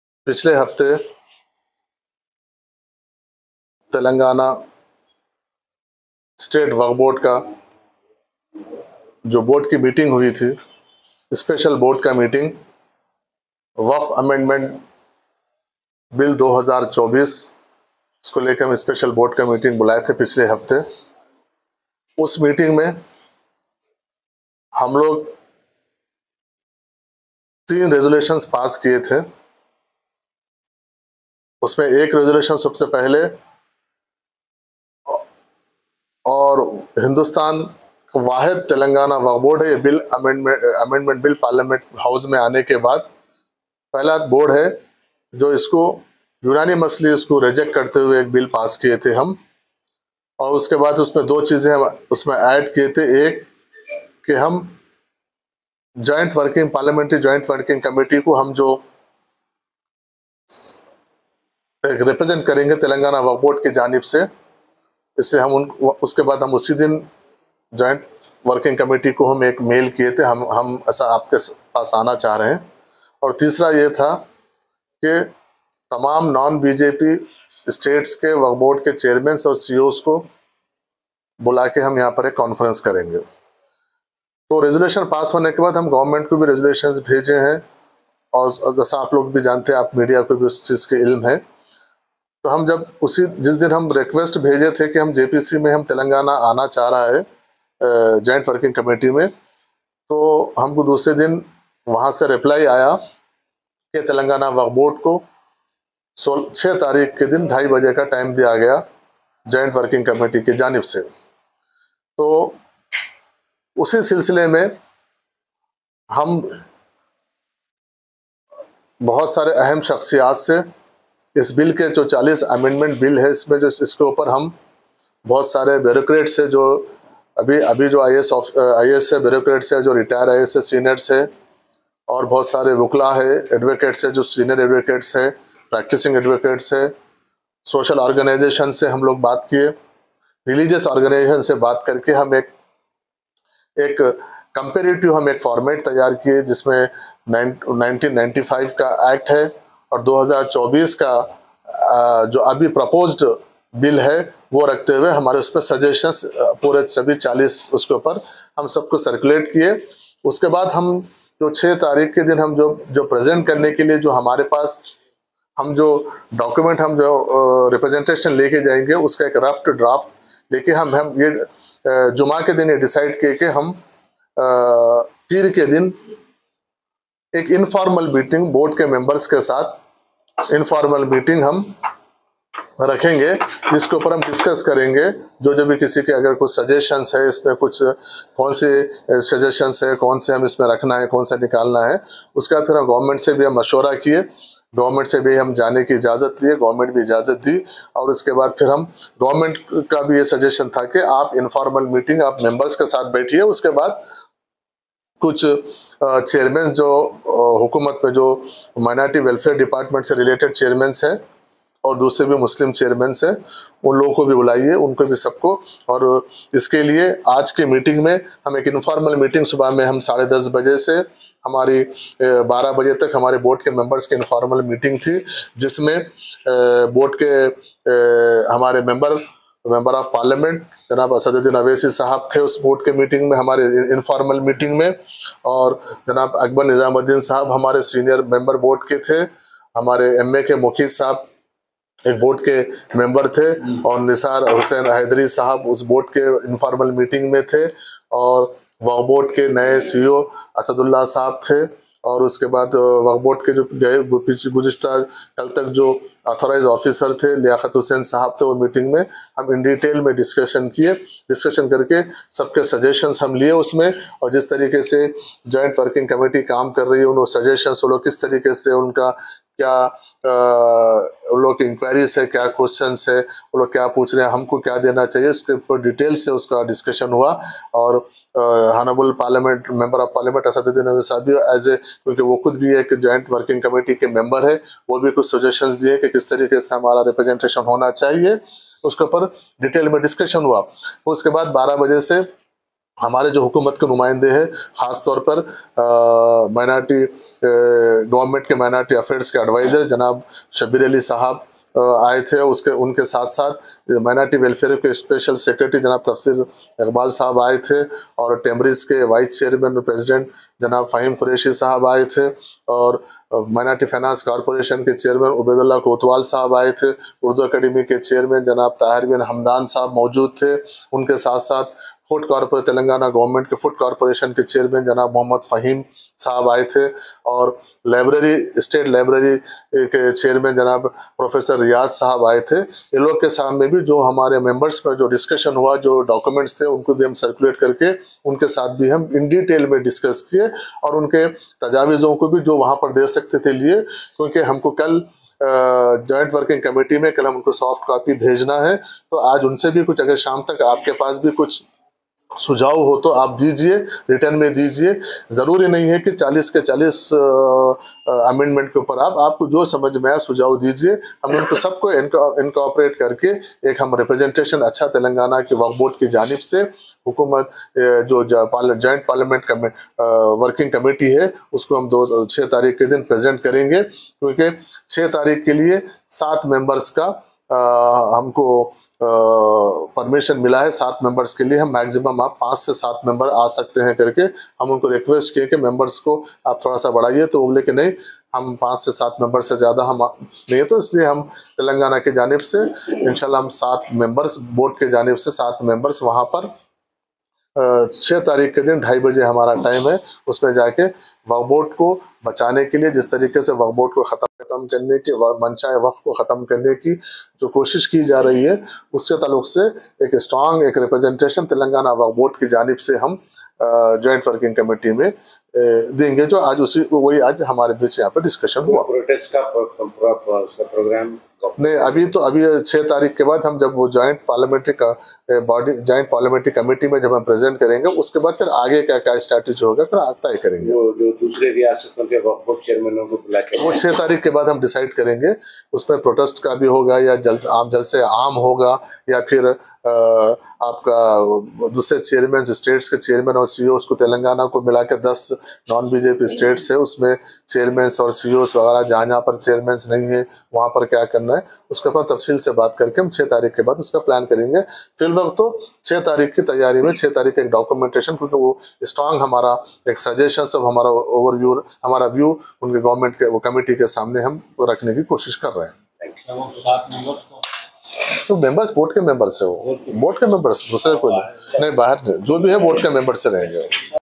T.S WAQF BOARD KE 7 ARKAAN WAQF AMENDMENT BILL 2024 KE KHILAF NUMAINDGI KARENGE
TELANGANA STATE WAQF BOARD KE 7 ARKAAN WAQF AMENDMENT BILL 2024 KE KHILAF JOINT WORKING COMMITTEE SE NUMAINDGI KARENGE..

(NEWS14..NAYYER)